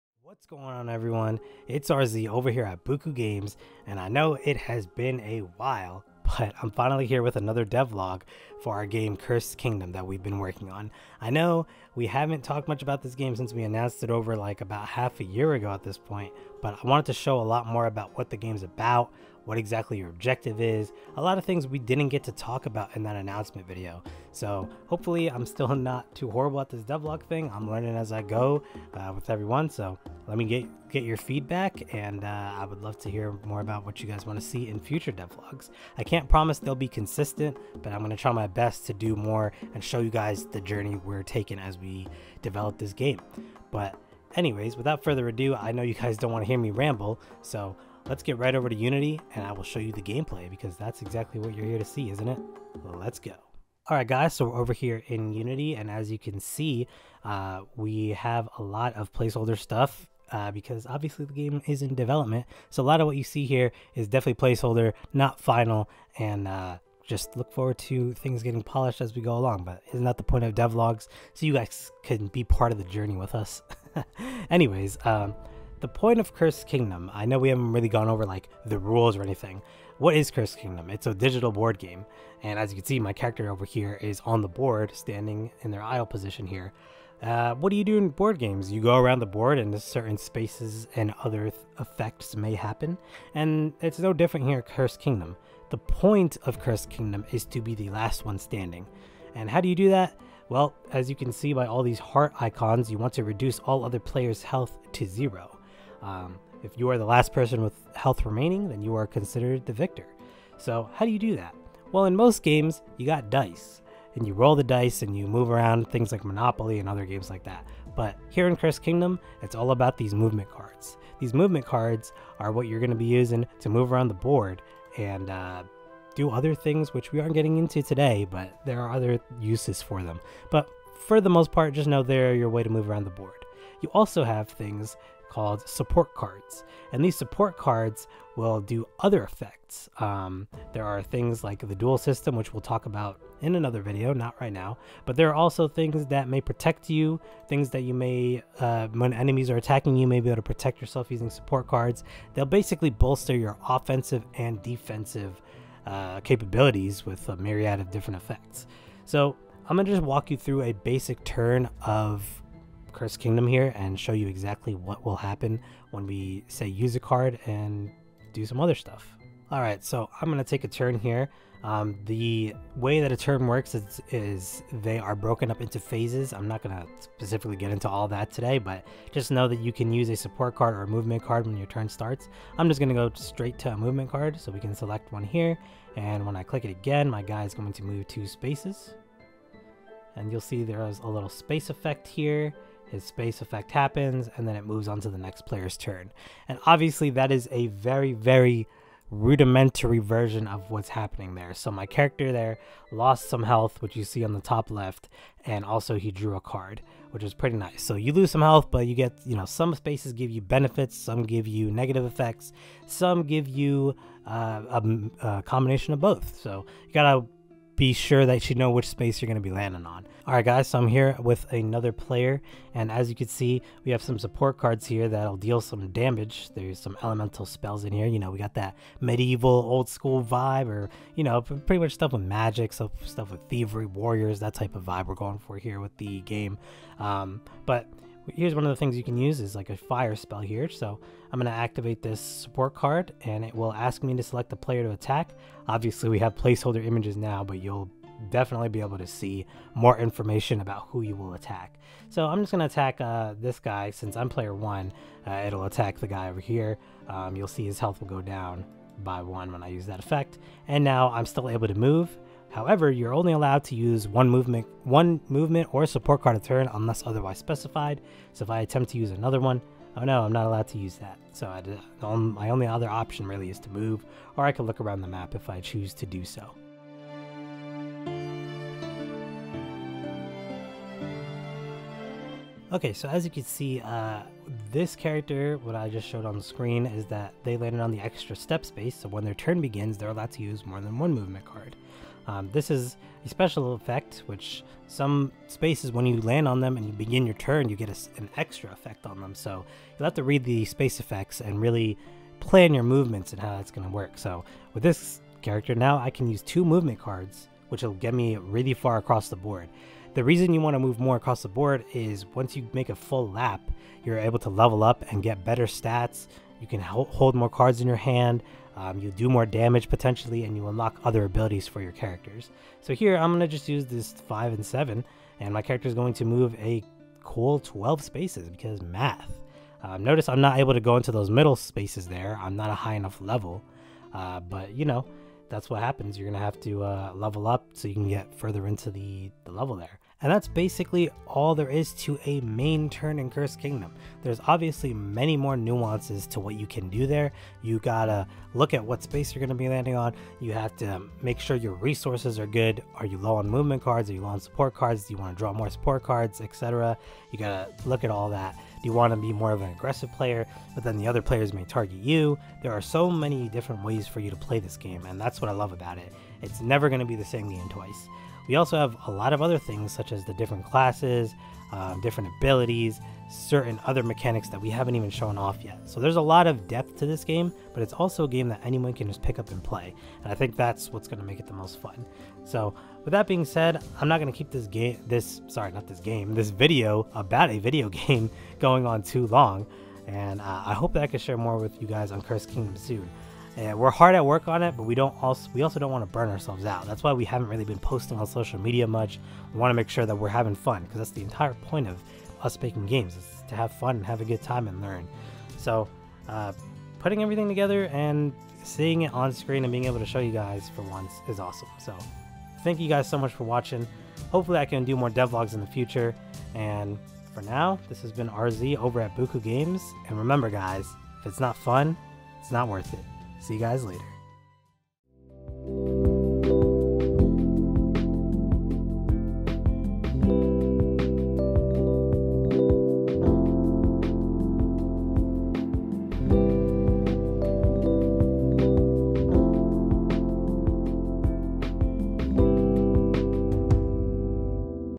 Cursed Kingdom Devlog # 1 - What's the game about!?
Cursed Kingdom is the next game being developed by BukuGames! In this devlog RZ talks about what the game is all about. Objectives such as eliminating other players, movement around the board and support cards are discussed.

*Cursed Kingdom is currently in development and any gameplay shown in this video is subject to change by release*